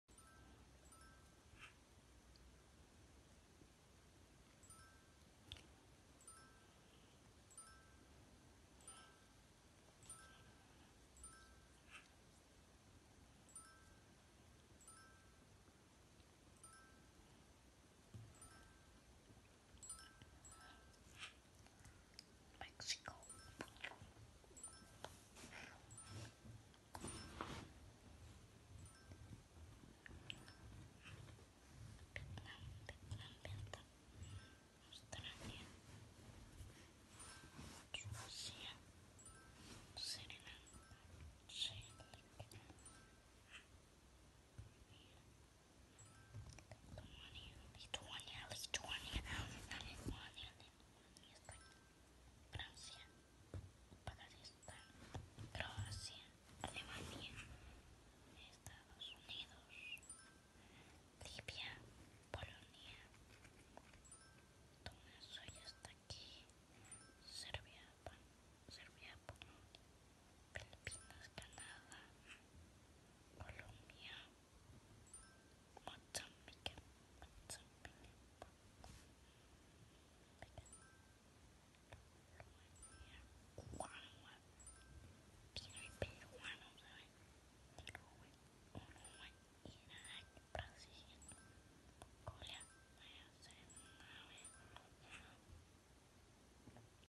Mateo y el reto de las banderas del mundo
Mateo es un niño de 9 años de edad, reside en San Salvador, le encanta la geografía, fue decisión propia instalar una aplicación móvil con las banderas del mundo y de una manera sorprendente ha logrado aprenderlas.